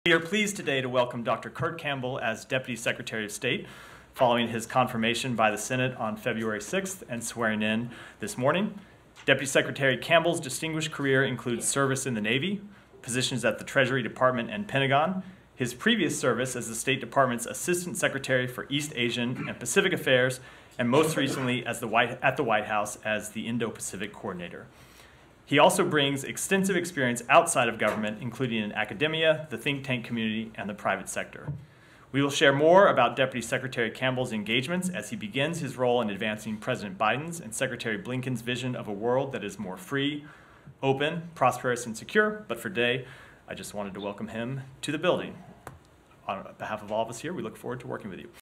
Welcoming Deputy Secretary of State Campbell | February 12, 2024
Dr. Kurt Campbell began his tenure as the 22nd Deputy Secretary of State today after being sworn in this morning. We welcome him back to the State Department following his most recent public service as Indo-Pacific Coordinator at the @WhiteHouse. — Spokesperson Matthew Miller during the Department Press Briefing on February 12, 2024.